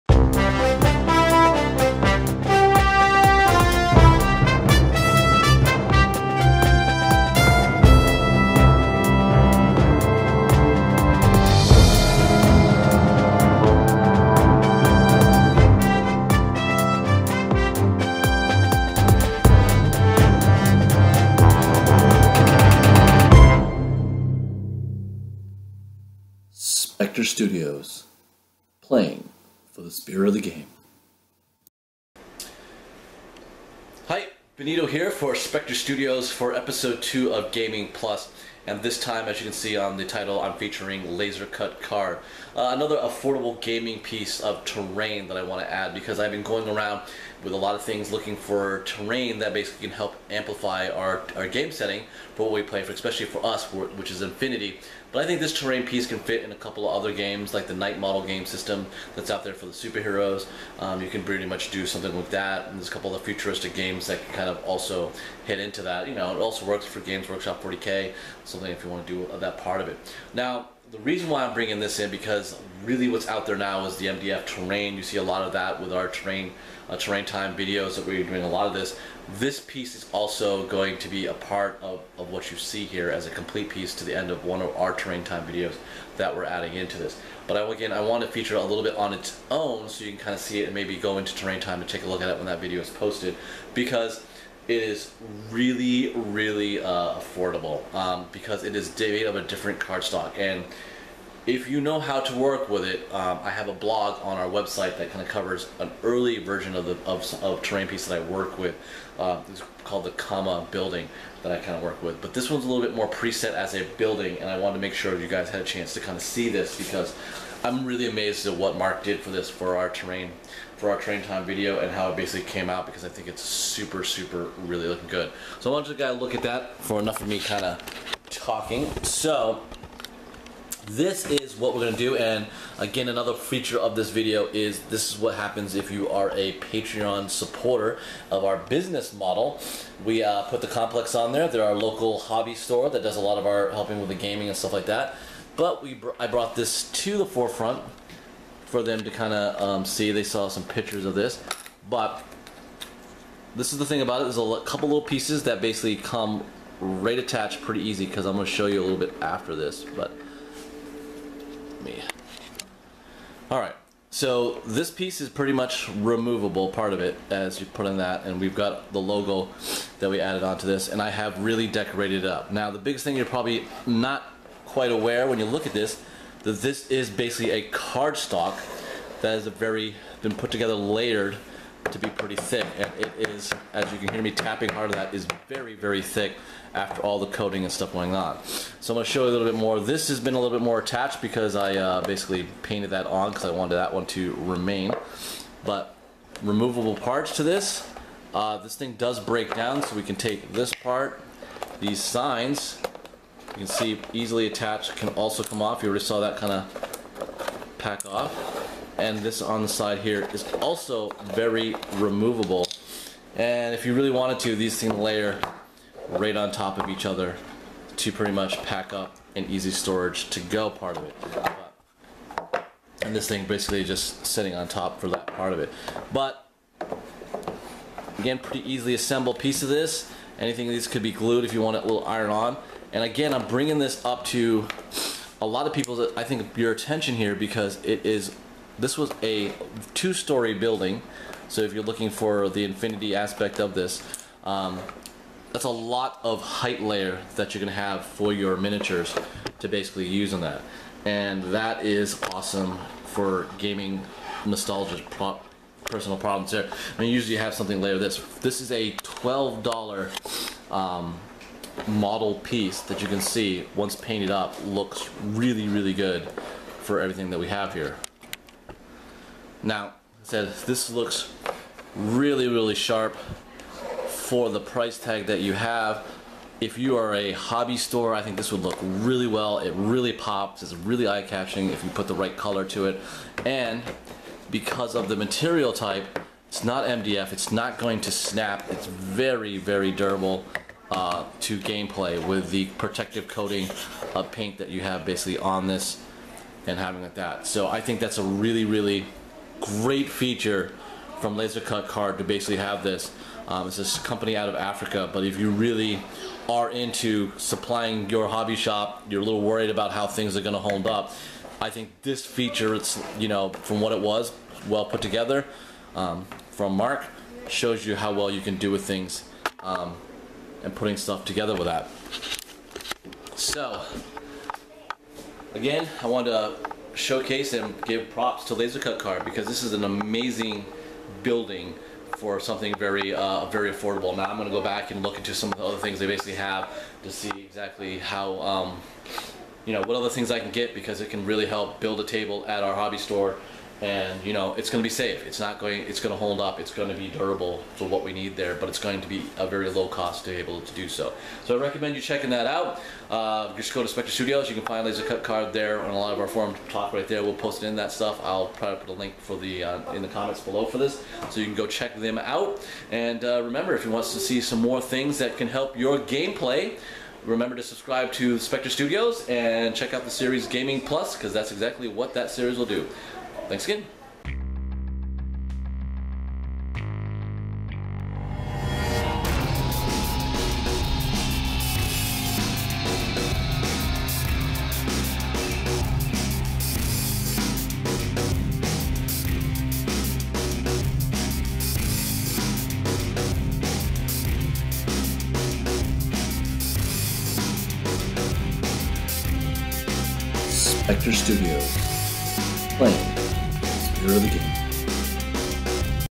Gaming Plus: Episode 2
Benito showcases a building from LaserCutCard a company out of Africa. A very affordable terrian piece check his thoughts